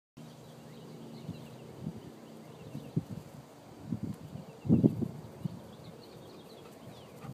My Movie
We here at the Emerald Coast Golden Retriever Rescue recently came across a very special labrador through our work with other area rescues that don't exactly fall under our primary focus with goldens   We invite you to visit this section of "honorary goldens" to meet Maverick a 3 year old Chocolate Lab who is looking for a forever home.  Could it be you?  Come visit us at ECGRR.com and learn more about Maverick...